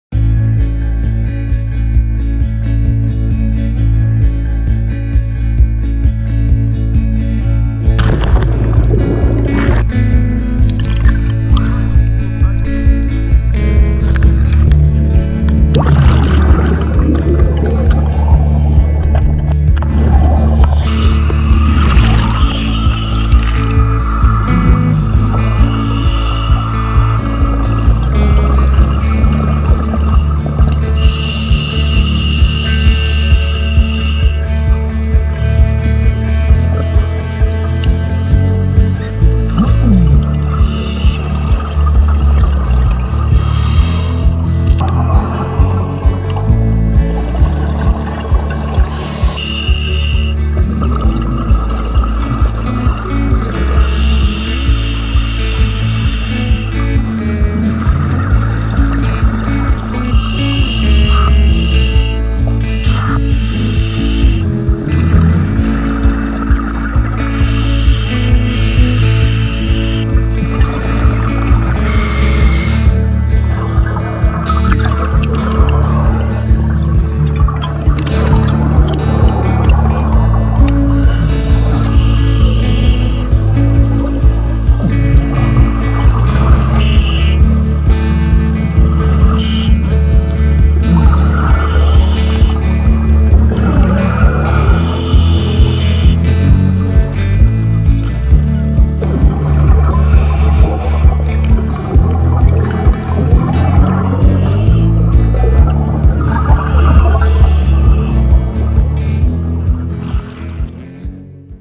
Diving in Malta - MV Karwela - Xatt l-Ahmar
The islands of Malta offer a versatile opportunity to divers who wish to experience various forms of wreck diving. In Gozo one can find MV Karwela located at Xatt l-Ahmar. The beauty of this wreck is spectecular